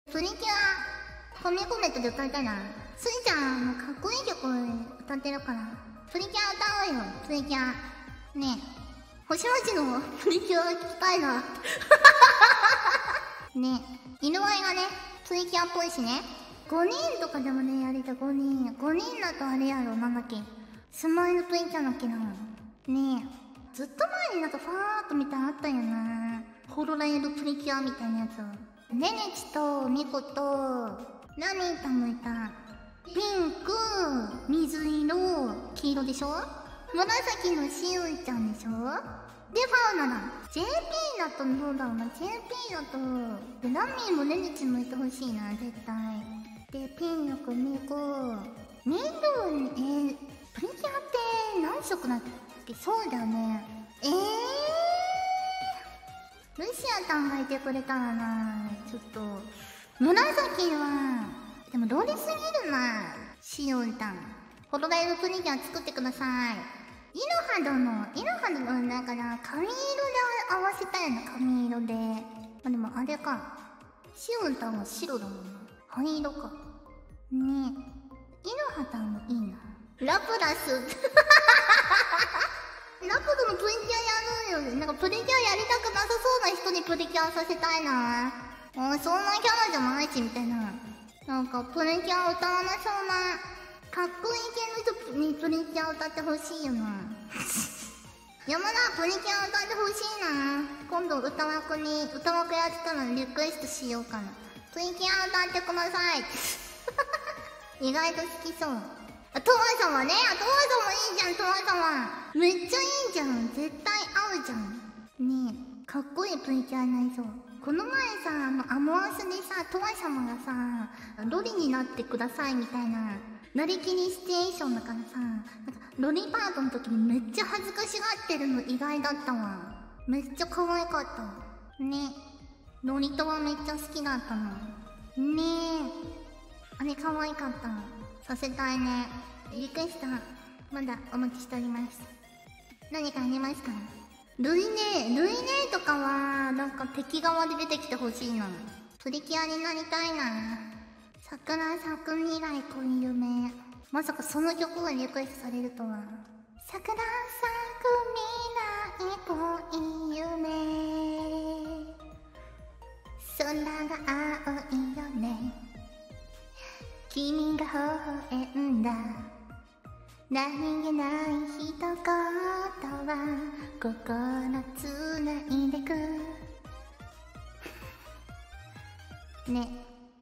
ホロライブメンバーでプリキュアがやりたくて色々と妄想するみこちｗ【ホロライブ切り抜き/さくらみこ/星街すいせい/常闇トワ】
▼元動画▼